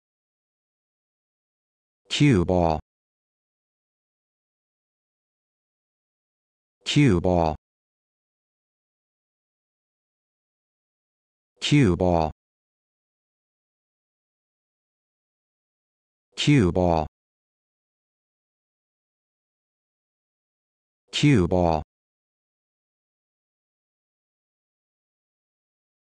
How to Pronounce Cueball
This video shows you how to pronounce Cueball